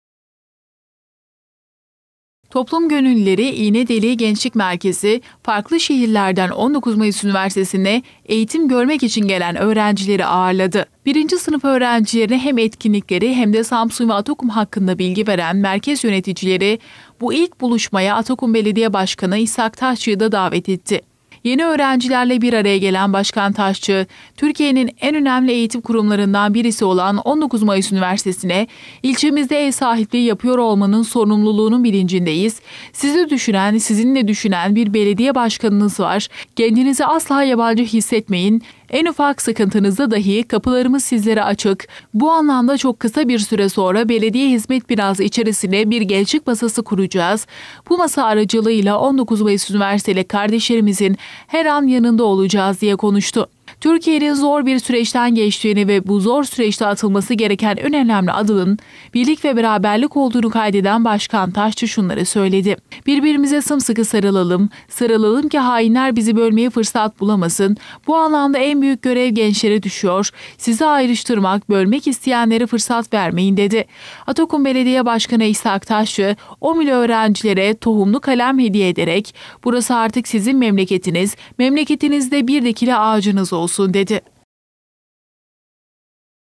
'SİZİ DÜŞÜNEN BİR BAŞKANINIZ VAR'
İğne Deliği Gençlik Merkezi'nde yeni OMÜ’lülerle bir araya gelen Atakum Belediye Başkanı İshak Taşçı, "Sizi düşünen, sizinle düşünen bir belediye başkanınız var" dedi.